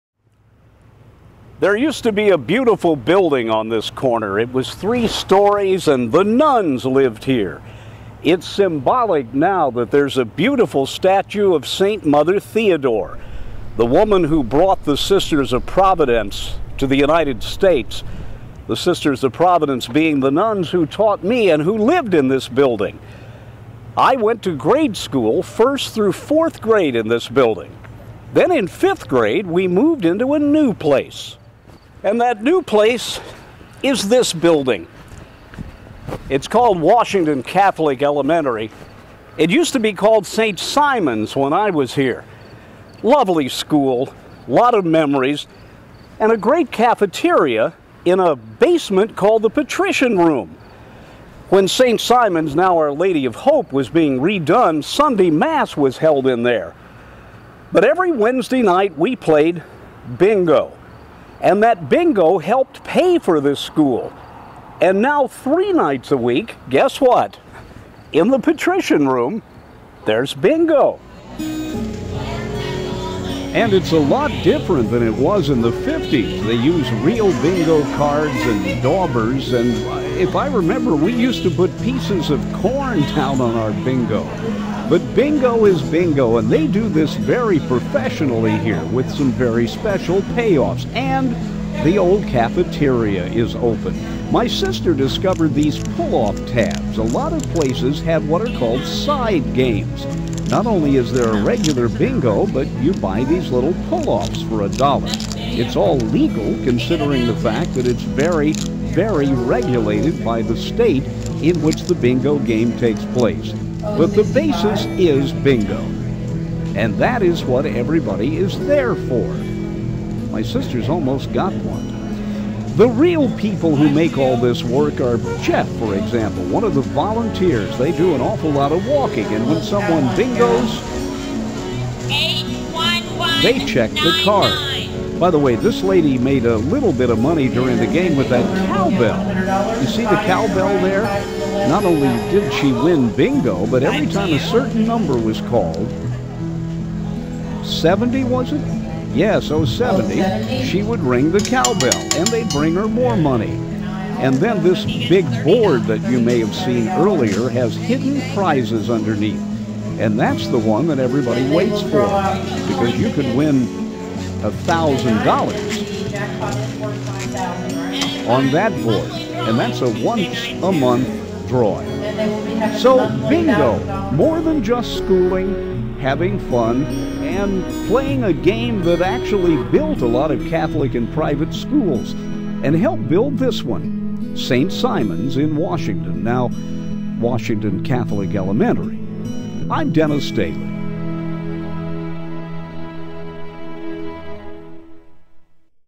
THE BIG TRIP: Memories of BINGO!
DENNIS DAILY continues his series of videos prepared during his recent 5,000-mile trip around two-thirds of the country. Today he reports on a very special place where he first played BINGO in the 1950s.